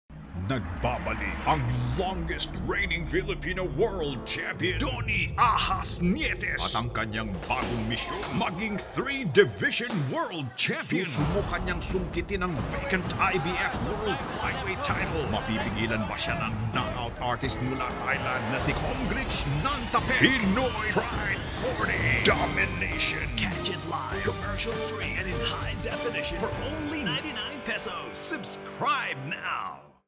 Pinoy Pride 40: Domination
It's Donnie “Ahas” Nietes versus Komgrich Nantapech for the IBF World Flyweight supremacy this Saturday, April 29, 6PM. Enjoy the live, commercial-free and high definition broadcast on SKY Sports Pay-per-View for only PHP 99.